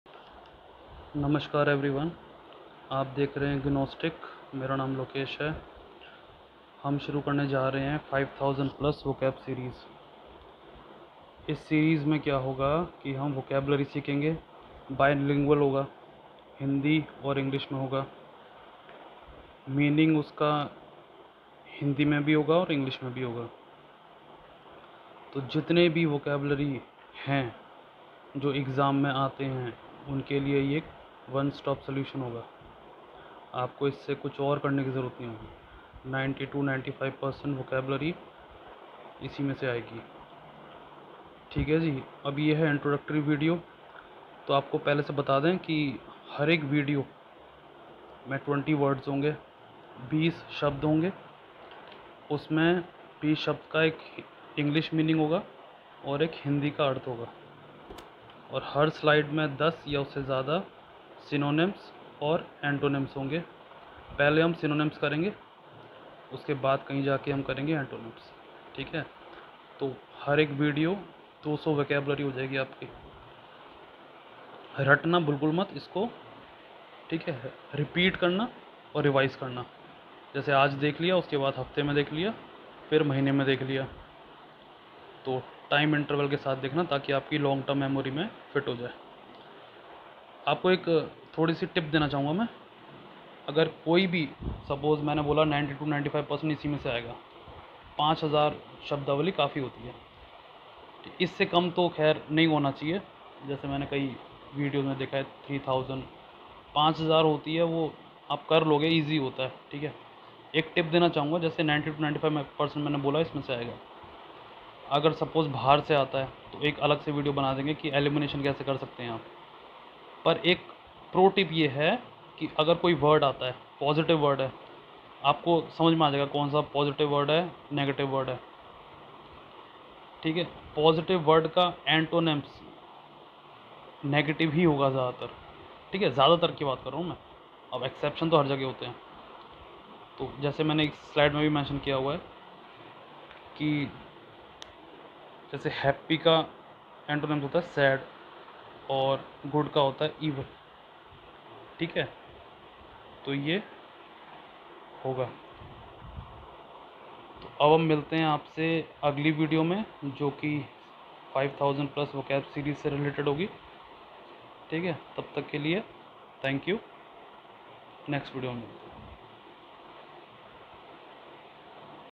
Introduction || 5000+ Vocab Series || Letter A - Z synonyms antonyms #200 words || Bilingual
Vocabulary Series Letter "A to Z" (Vocabulary playlist) having Synonyms and Antonyms with Hindi meaning For all Government Examination, College Entrance Test, University Exam, Competitive examination and most importantly for Self improvement.

Do not memories instead "Repeat and Revise"